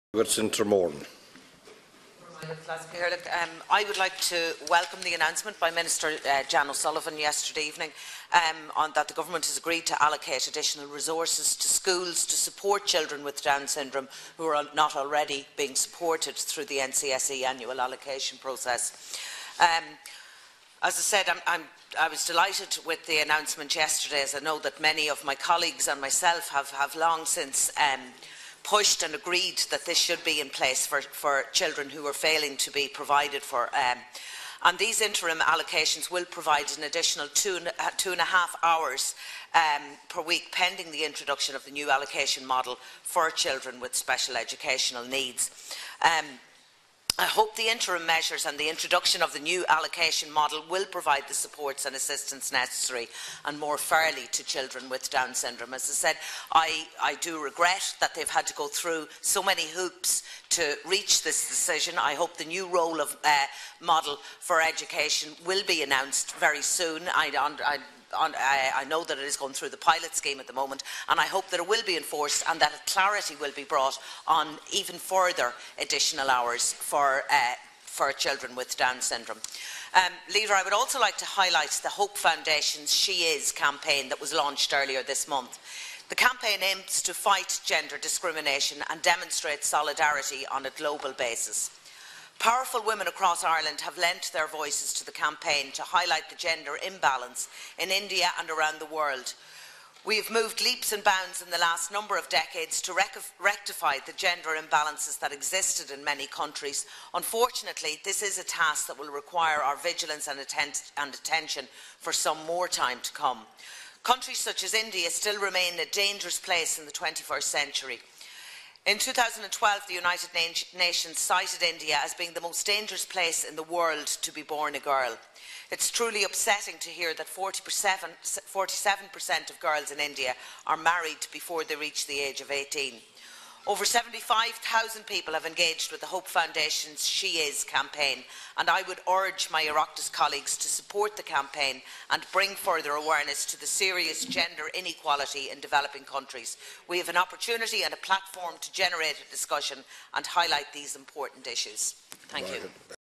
Order of Business - 25th March 2015
Speaking in Today's Order of Business on Additional Resources Made Available to Support Children with Down Syndrome & The Hope Foundationan's SHEis Campaign